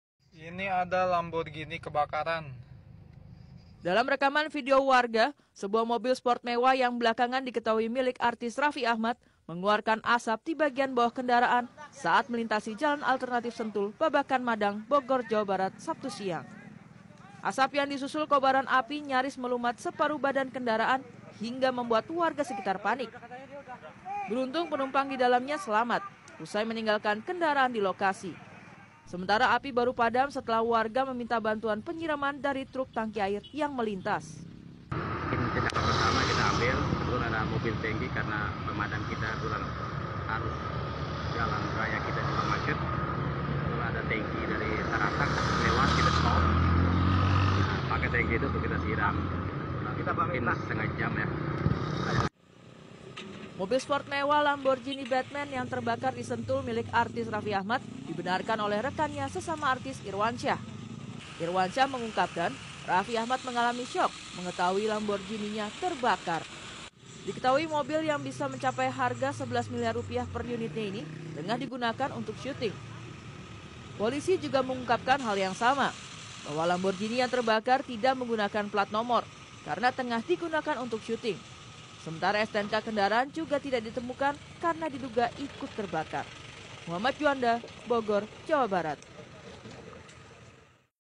Mobil Raffi Ahmad terbakar.!!
Mobil mewah raffi ahmad terbakar, mobil seharga 13M Lamborghini Batman milik raffi ahmad terbakar di dekat pintu masuk a
Sentul